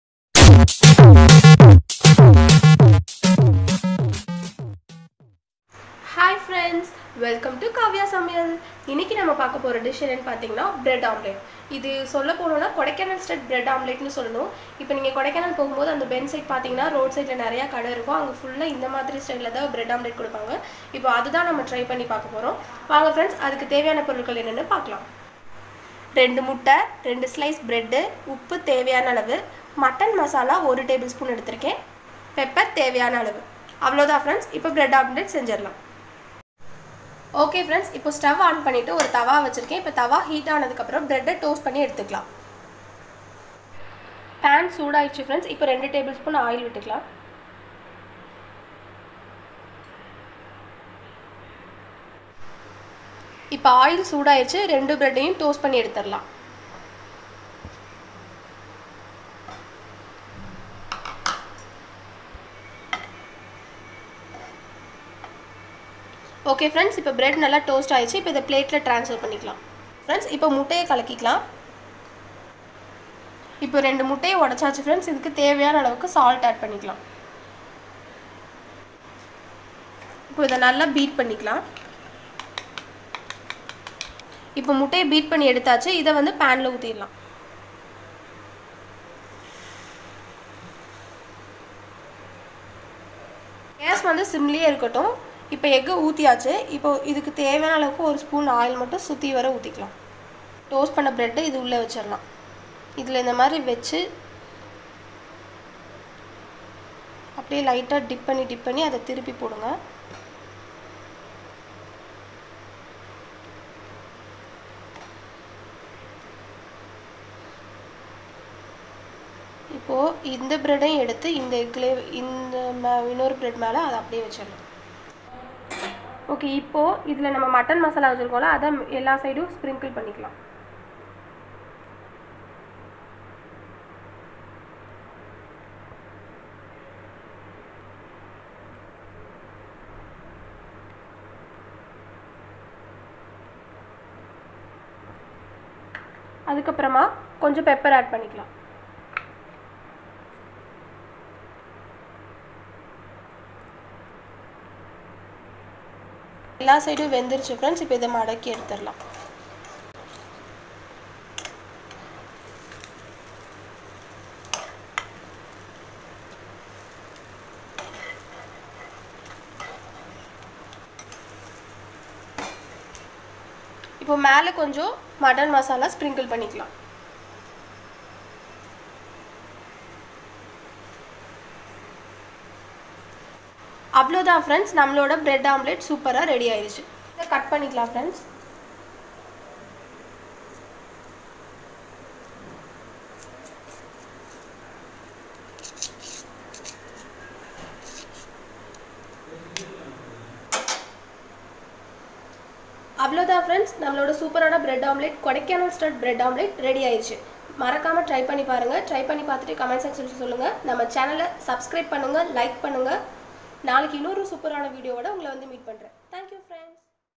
Kodikanal style Bread omlet | how to make bread omlet in tamil
In this video, we will see how to make bread omlet in tamil..